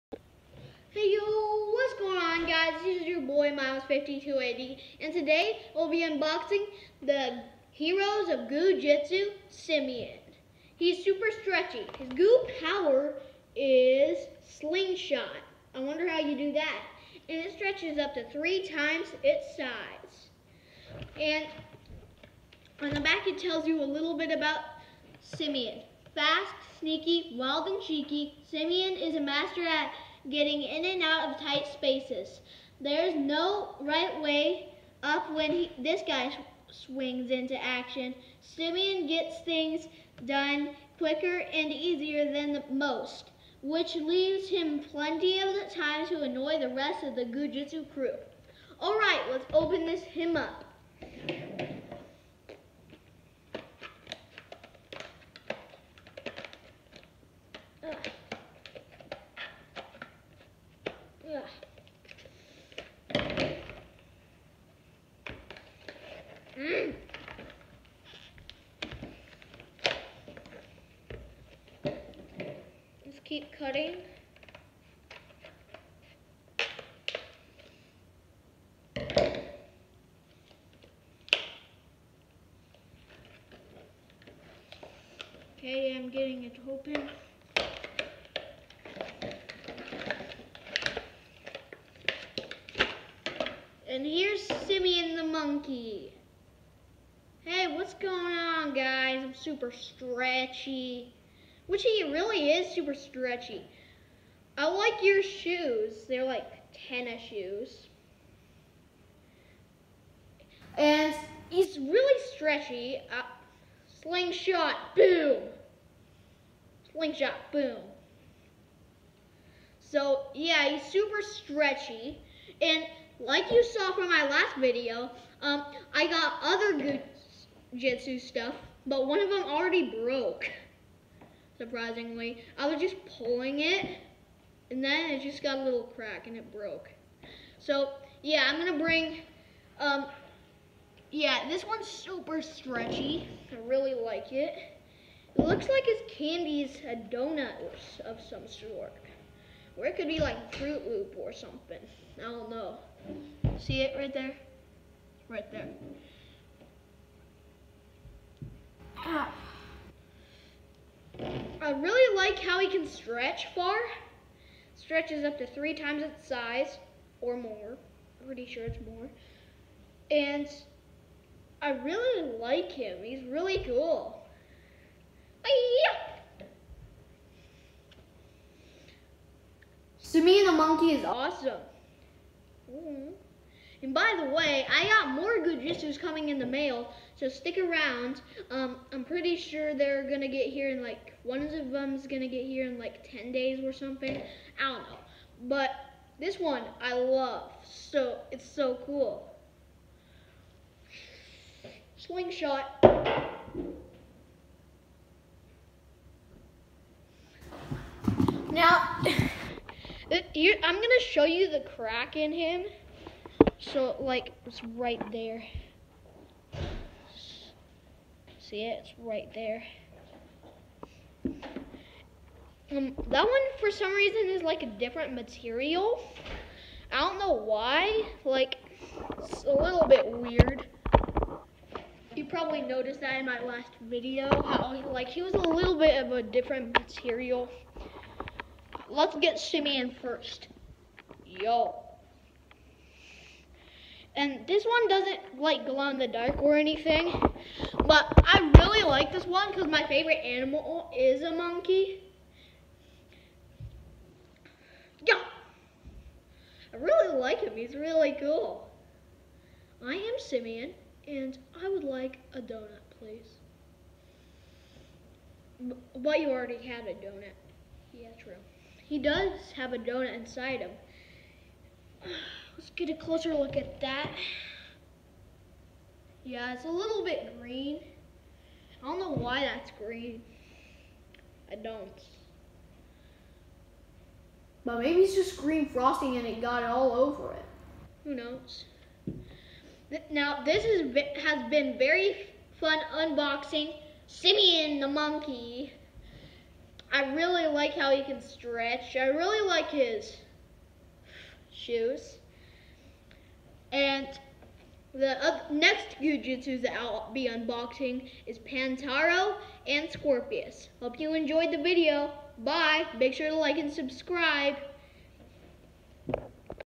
Unboxing Goo Jit Zu Simian
Watch Miles 5280 unbox Simian. A very cool addition to the Goo Jit Zu collection.